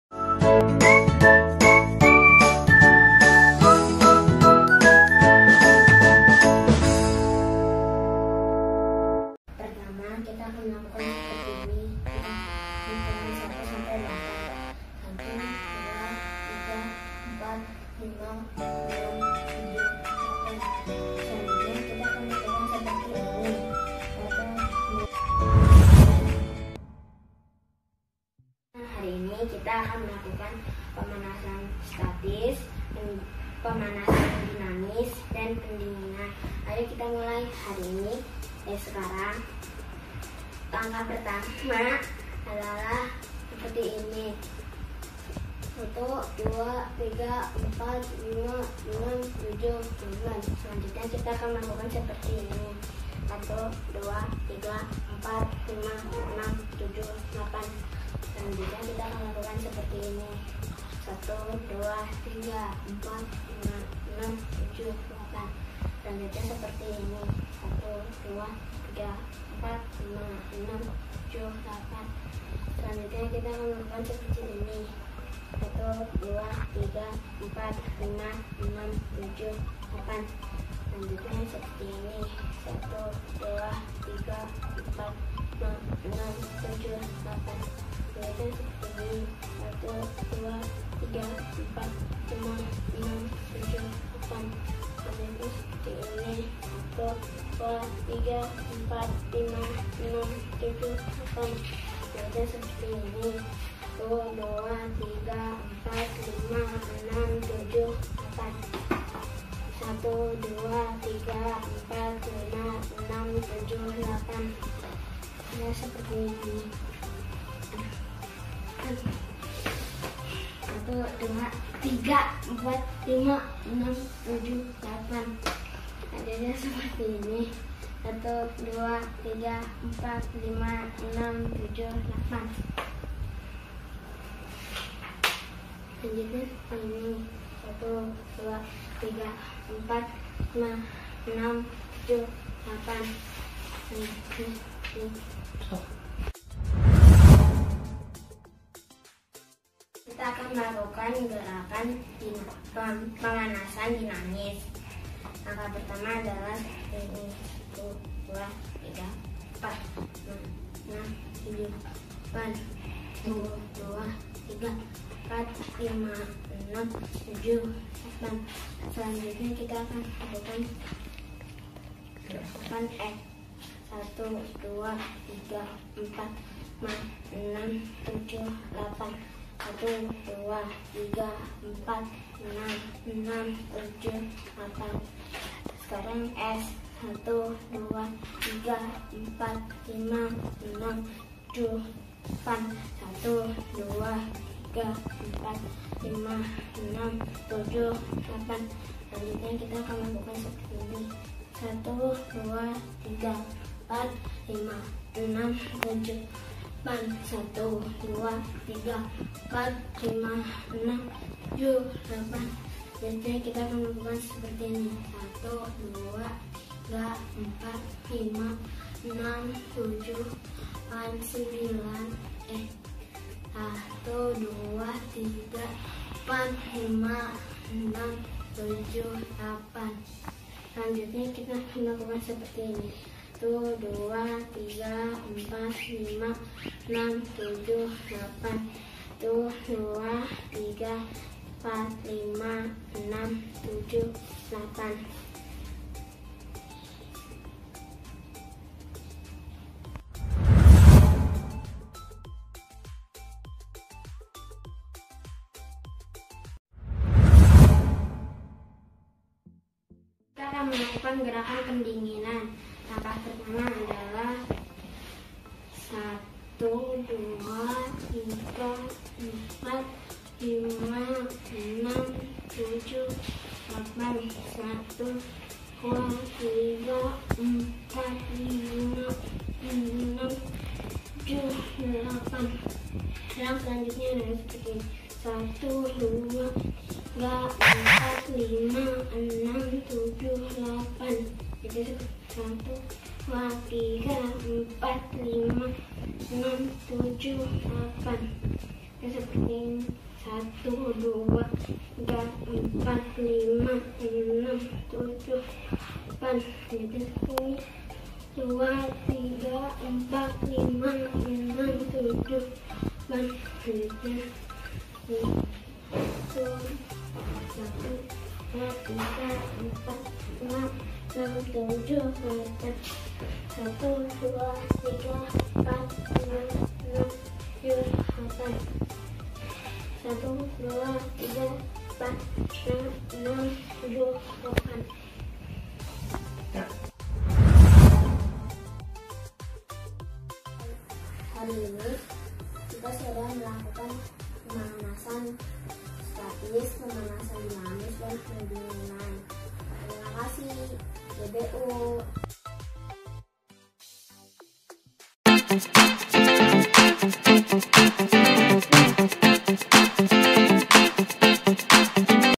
Lihat Kak Cia Ajarkan Pemanasan Statis dan Dinamis Yuk!!! Belajar Senam dengan Cia!!!
Konten keluarga kita. Kali ini by Kak Cia! Olahraga yuk olahraga.